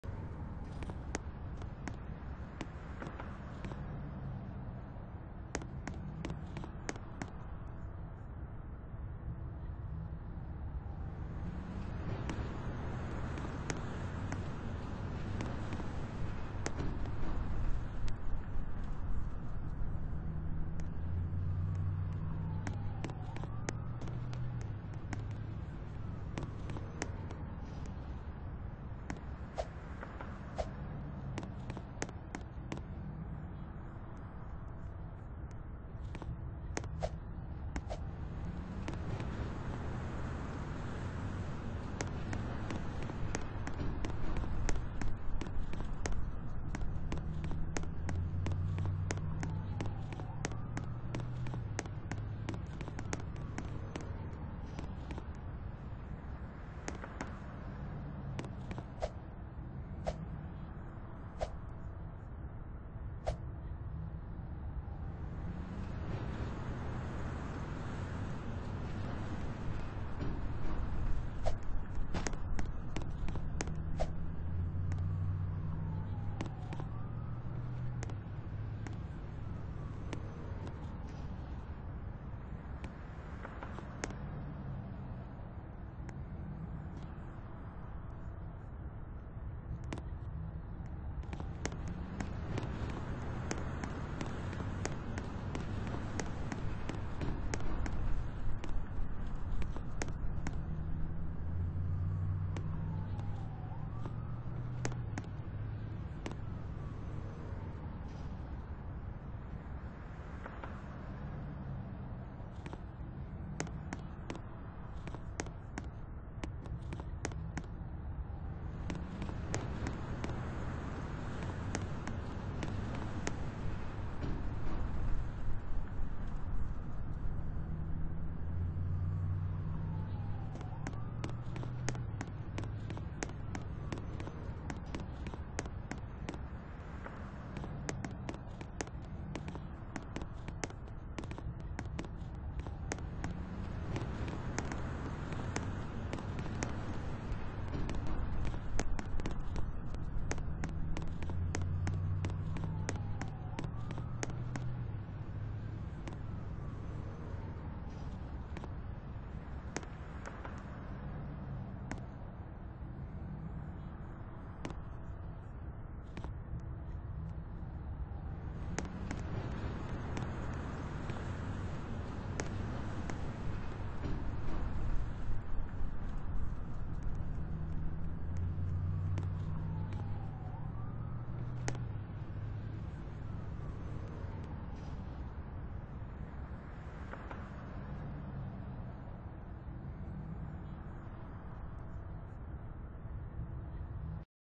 preparations for hurricane Jared Hitting VTimes square.
This Footage is From Tom-TV News anyways a real dangers new hurricane Jared that is heading to the United States of north America that creepr is the President of and VTimes Sqaure anyways it could flood in VTimes Sqaure and the place that could hit but creepr is not sure what could be happened for more info about this hurricane pls contact @CreeprDuDE89 he says he thinks the hurricane is expected to hit VTimes square during the weekend.